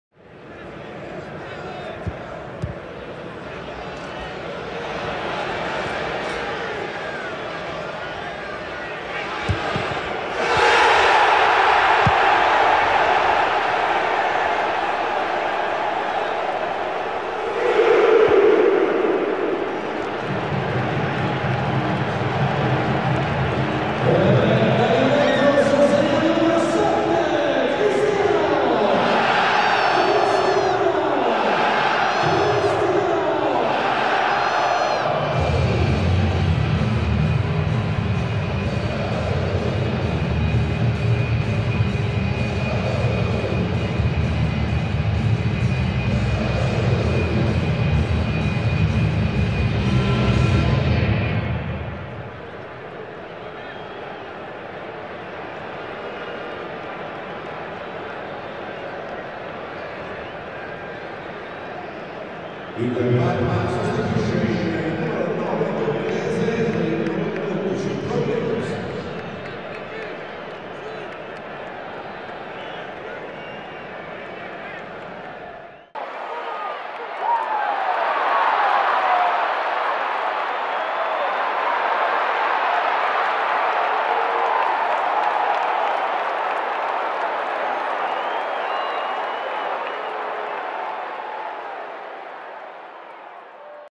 Ronaldo Opens the Scoring at the Juventus Stadium! | Juventus 2-1 Parma | Top Moment | Serie A TIM
Cristiano Ronaldo scored for a seventh successive Serie A game as his brace gave leaders Juventus victory at home to Parma | Serie A